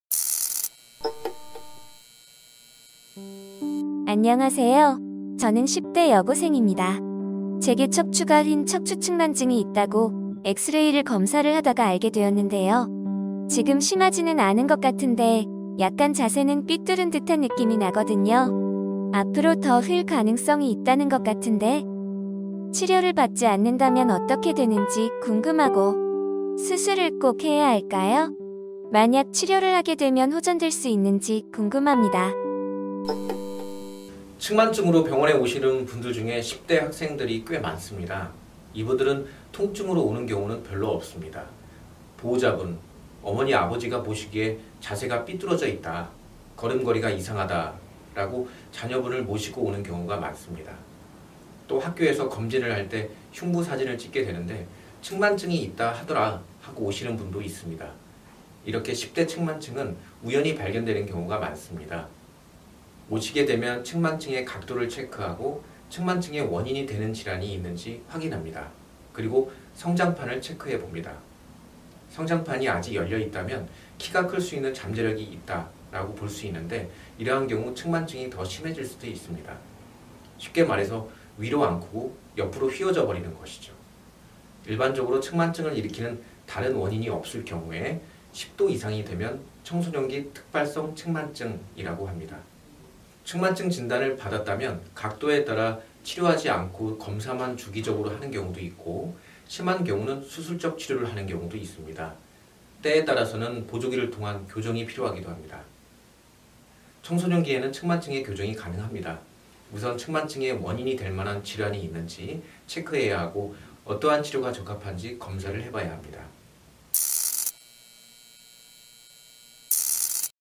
[사연읽어주는의사] 10대 척추측만증 수술해야 하나요?
[오늘의 사연]
안녕하세요 저는 10대 여고생입니다.
제게 척추가 휜 척추측만증이 있다고 x-ray를 검사를 하다가 알게 되었는데요
지금 심하지는 않은 것 같은데 약간 자세는 삐뚫은 듯한 느낌이 나거든요.
앞으로 더 휠 가능성이 있다는 것 같은데
치료를 받지 않는다면 어떻게 되는지 궁금하고수술을 꼭 해야 할까요???
만약 치료를 하게 되면 호전될 수 있는지 궁금합니다.